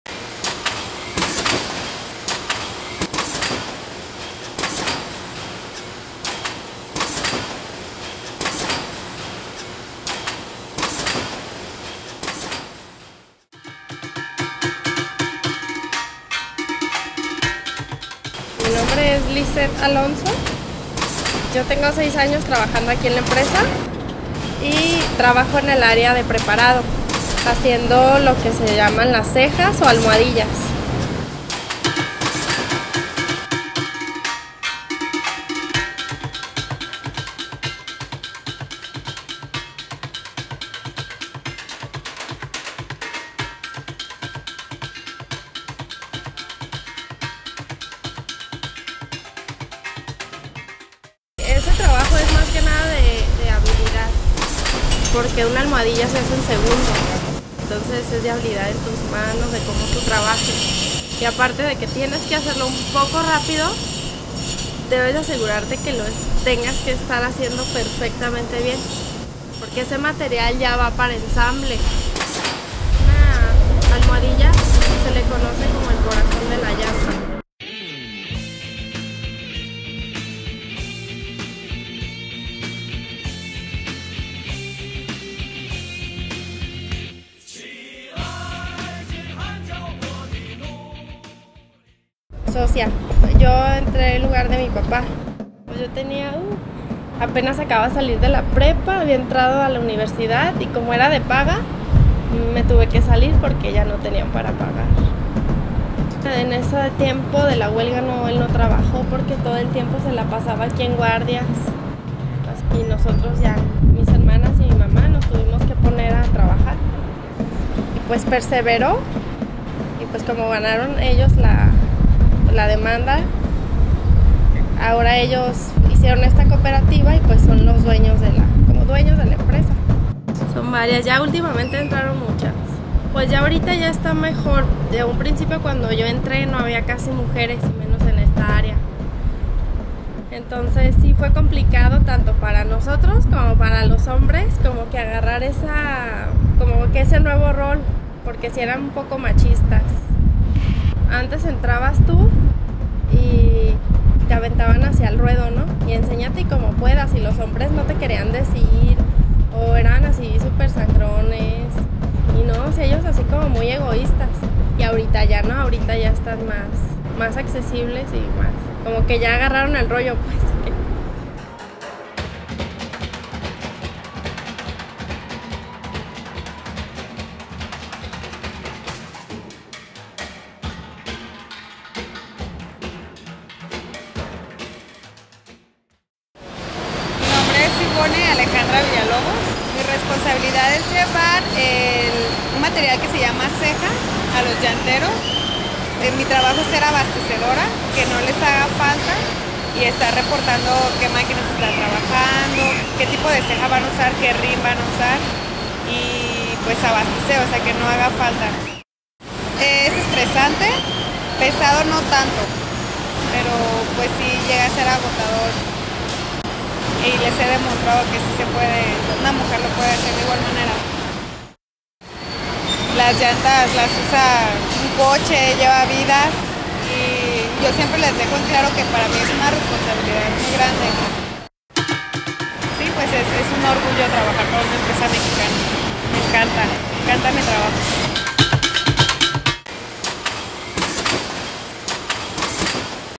Mujeres llanteras en TRADOC
Redacción La Coperacha
Ciudad de México // 21 de junio de 2016

La cooperativa Trabajadores Democráticos de Occidente (Tradoc), se ubica en El Salto, Jalisco, en un predio de 64 hectáreas.

En sus inicios, y luego de tres años de huelga, los trabajadores del entonces sindicato de Euzkadi arrancaron legalmente la fábrica a la llantera alemana Continental.

Actualmente es la única llantera mexicana y compite directamente con transnacionales como Michelin y Bridgestone.

En la planta laboran 120 mujeres, quienes desde hace 6 años comenzaron a cambiar el rostro a la cooperativa con trabajos que no pueden ser considerados exclusivos de hombres.

Varias mujeres son responsables del armado de las almohadillas, su labor es de tal importancia que a esas piezas les llaman, “el corazón de la llanta”.

De acuerdo a sus vivencias, las mujeres tuvieron que lidiar en un principio con situaciones difíciles, con machismos y egoísmos. Pero a partir de capacitaciones y educación en conjunto, mujeres y hombres trabajan en ambientes de respeto.

En Tradoc, mujeres y hombres producen a diario más de 20 mil llantas y las comercializan con éxito en EUA y México.